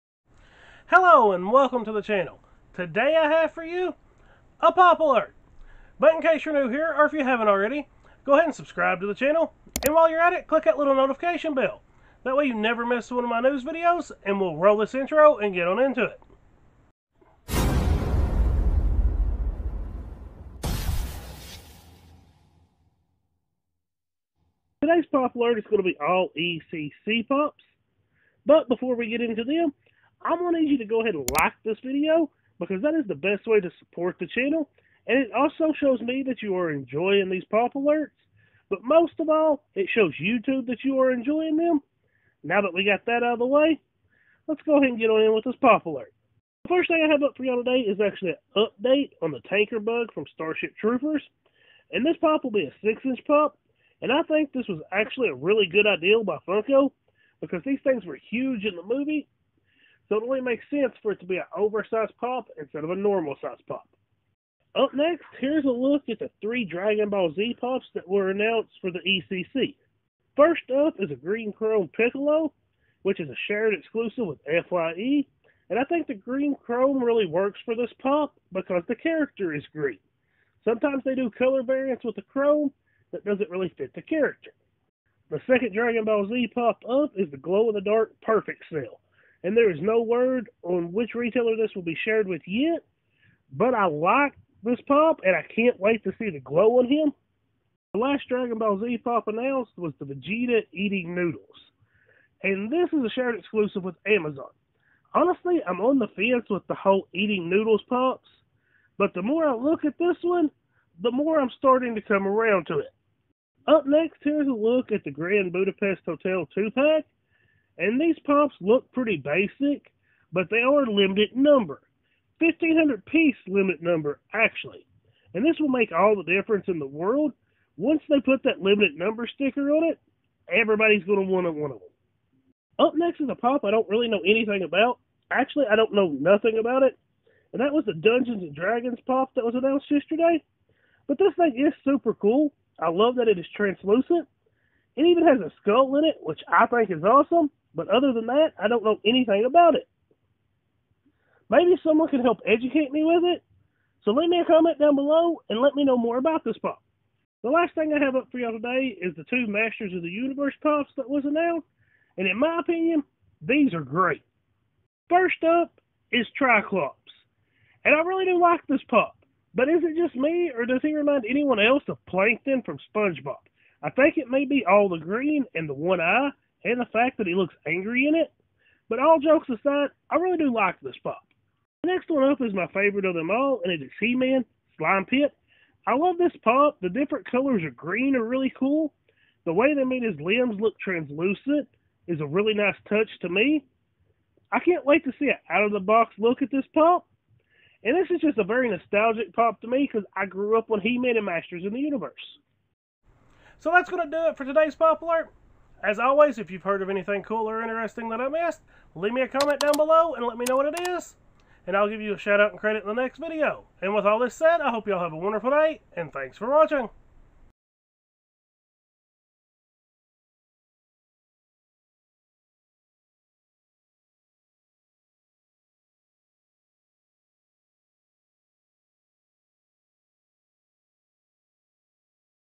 Funko Pop Alert February 12, 2020 | Funko News
Hello and welcome to the channel, this is episode 42 of the POP Alert!!! and is all about the ECCC pops announced for day 1.

Picture Credit:
@dis.pops & @pop.holmes on Instagram